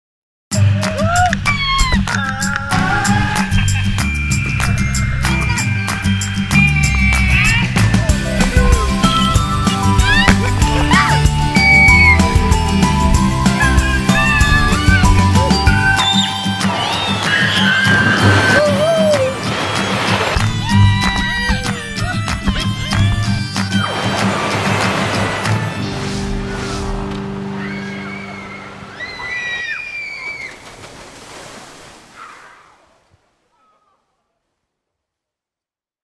Aqualand Moravia - nekonečná jízda
Nekonečná jízda v Aqualandu Moravia začíná! Léto 2015 se hlásí o slovo a s ním 20 tobogánů a skluzavek, v čele s gigantickou atrakcí Abyss a Boomerango! Nenechte si ujít tento jedinečný adrenalinový zážitek, který jinde ve střední Evropě nenajdete! K dispozici 12 bazénů včetně vlnového bazénu s novými režimy vlnobití! Rozsáhlý venkovní areál se slunečníky a lehátky. Římské lázně Forum Romanum Wellness s nabídkou 24 saun a procedur. Najdete nás jižně od Brna poblíž novomlýnských nádrží. Překrásný výhled na Pálavu právě u nás!  Užijte si léto s námi. Více informací na

Aqualand Moravia - největší a nejmodernější vodní svět v ČR!